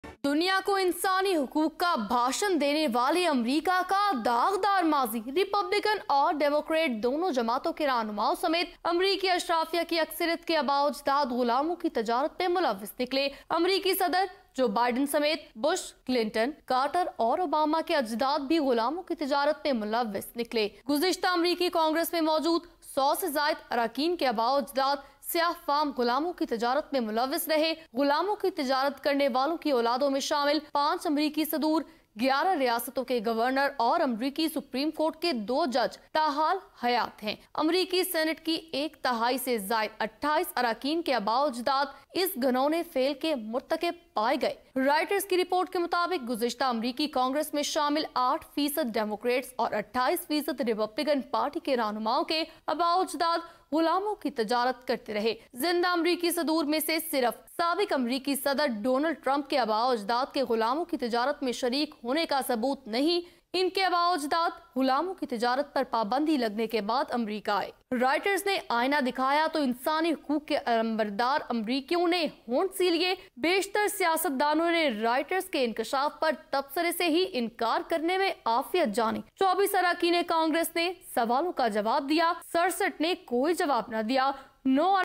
America's political elites have family links to slavery? || Special Report || ABNNEWS
More than 100 U.S. leaders – lawmakers, presidents, governors and justices  have slaveholding ancestors, a Reuters examination found.